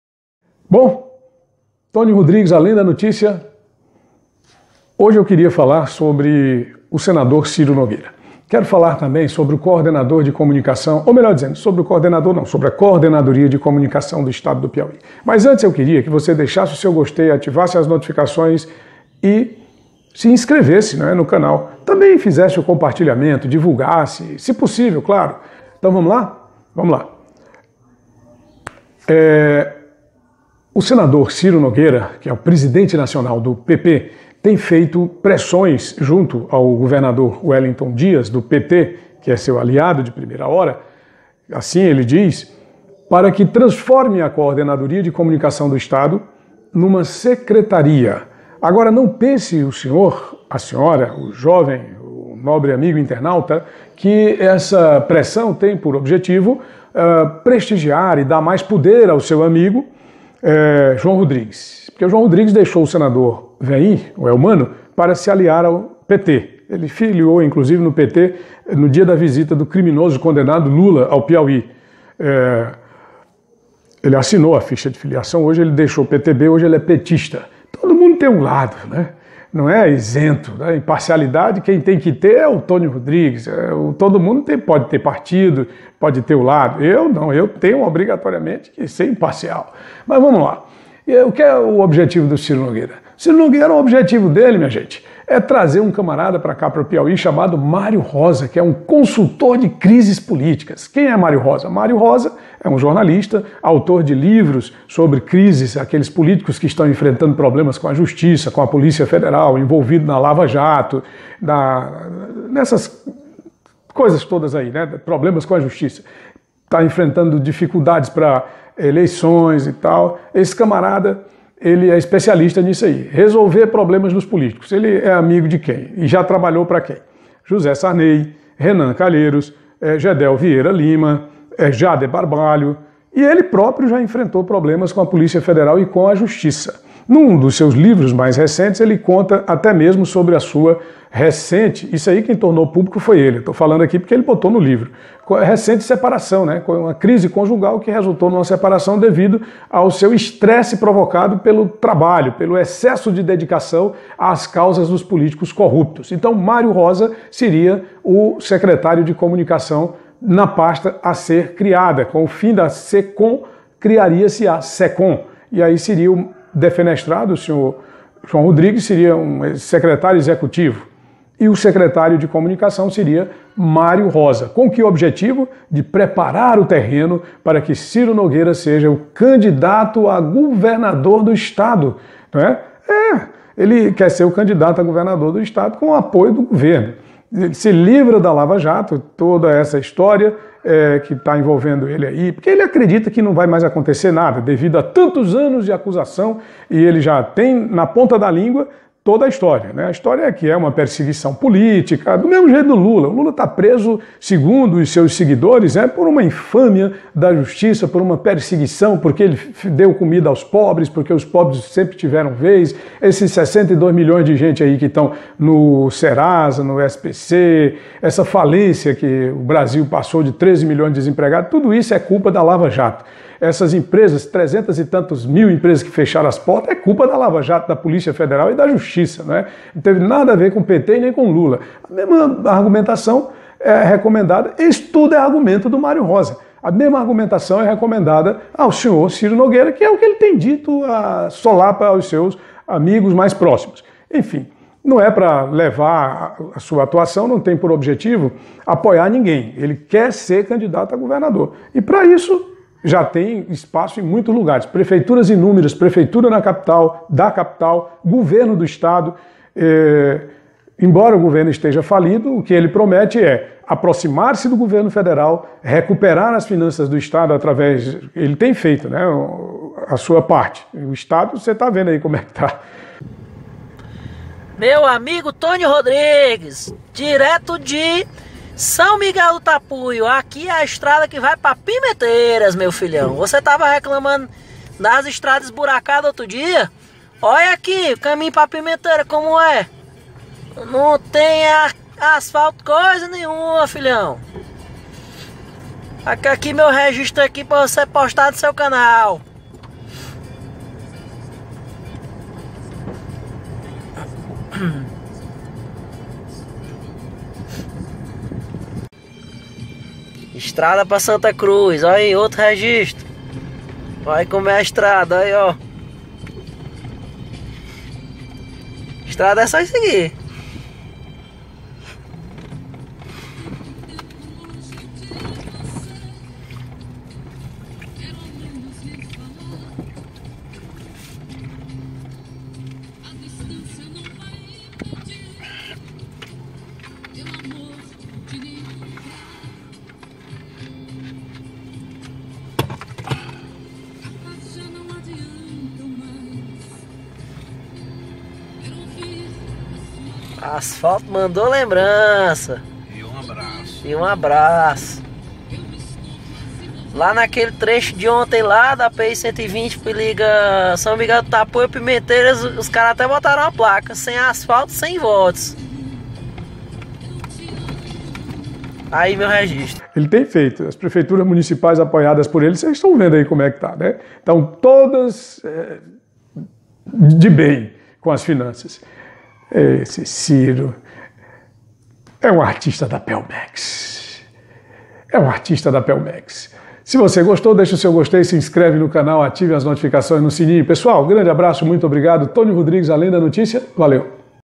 Mário Rosa no Controle do Centrão?
Ciro Nogueira, presidente nacional do PP, um dos partidos do “centrão”, sonha indicar jornalista e consultor de crises para secretaria de comunicação do Piauí, partido controlado pelo PT.

Mário Rosa na equipe do Vampiro Brasileiro (Bento Carneiro)?